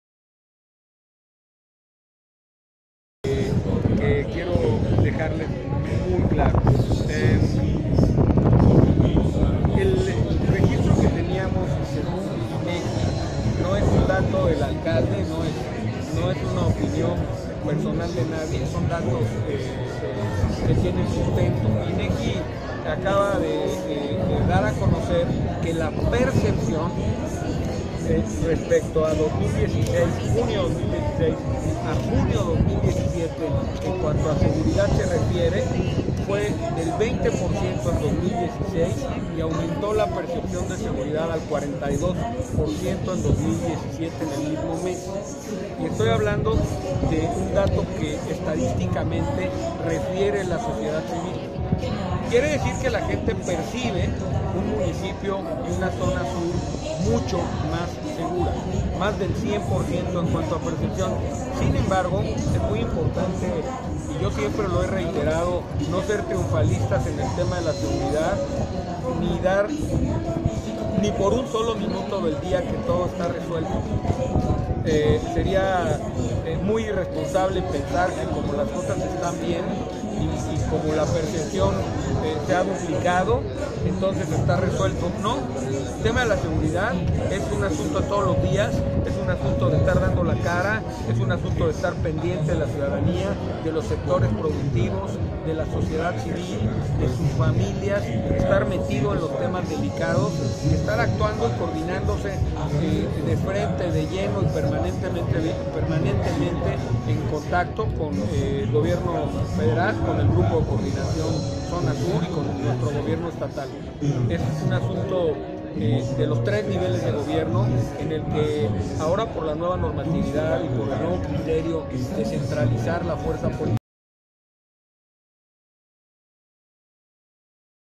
Afirma Alcalde de Cd. Madero que operativos nocturnos les han devuelto la seguridad
El Alcalde de Ciudad Madero, José Andrés Zorrilla Moreno indicó que la seguridad en su municipio va por buen camino mencionando que de acuerdo al INEGI, ahí aumento este tema un 42% en comparación del 2016.